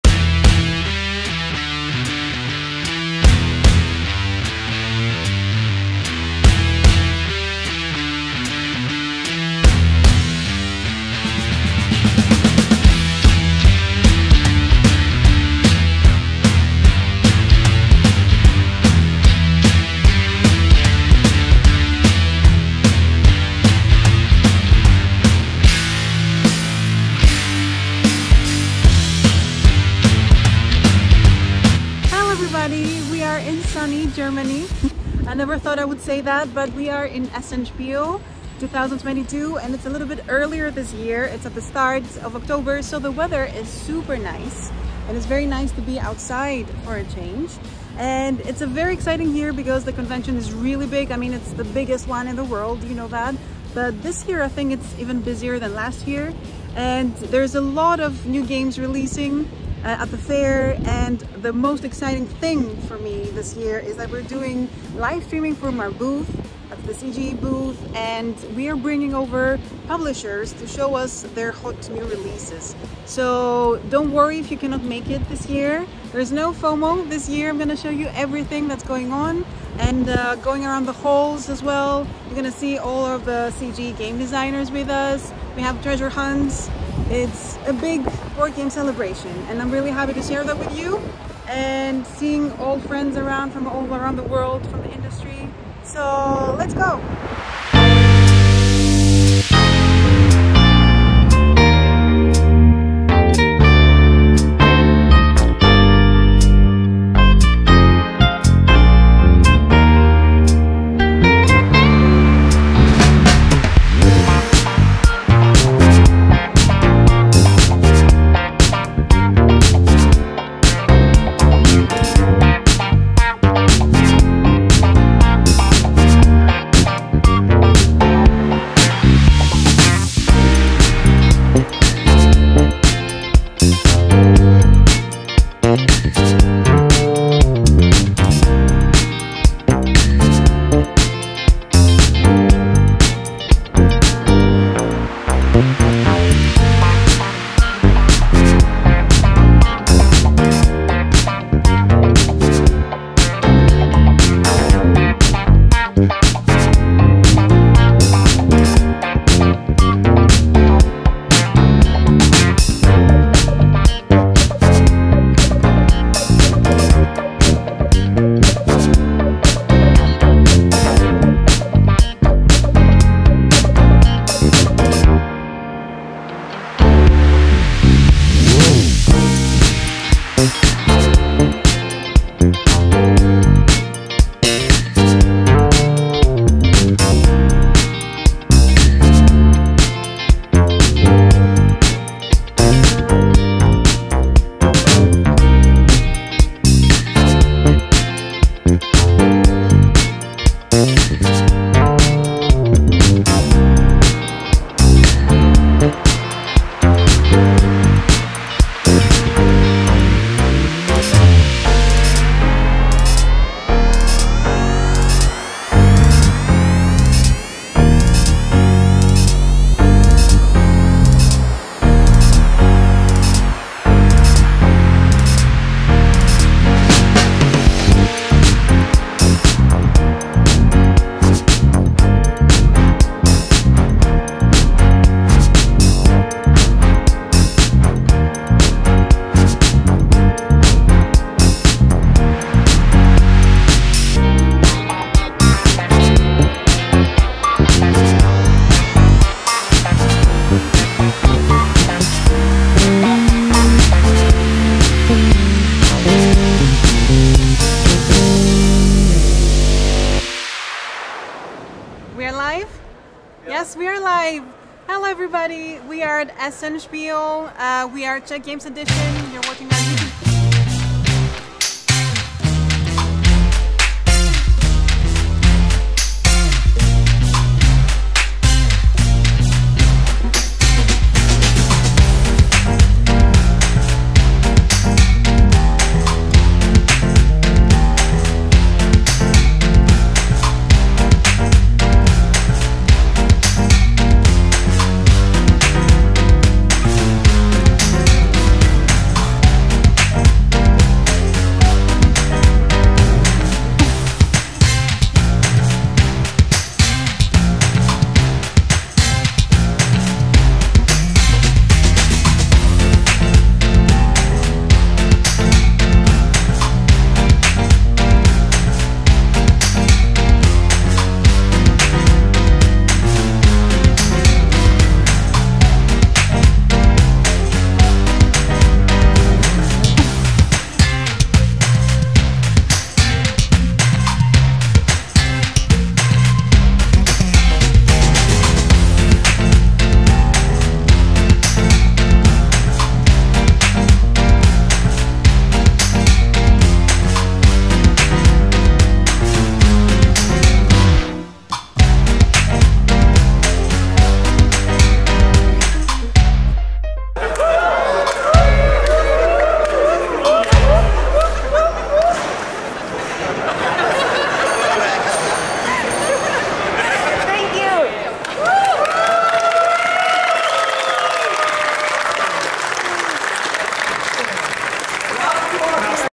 Essen Spiel '22 HIGHLIGHTS!
Essen Spiel 2022 was a blast! Travel around the convention with Eleni, see all the hot new releases of the fair and get the behind-the-scenes of how it feels to be at the biggest board game fair in the world!